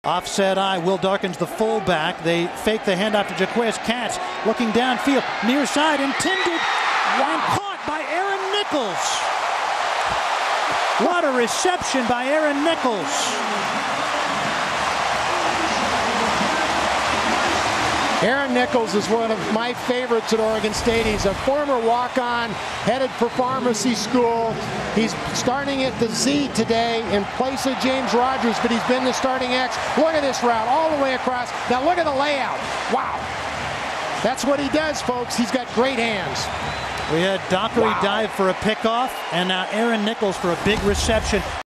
OSU Beavers Aaron Nichols receiver goes airborne vs. ASU
Senior OSU Beavers receiver Aaron Nichols (#46) lays-out to make an amazing 27 yard reception, from QB Ryan Katz (#12)- in the 1st QTR vs. The ASU Sun Devils.  The Beavers went on to victory in the 10/02 contest, 31-28, in their 2010 Pac-10 opener.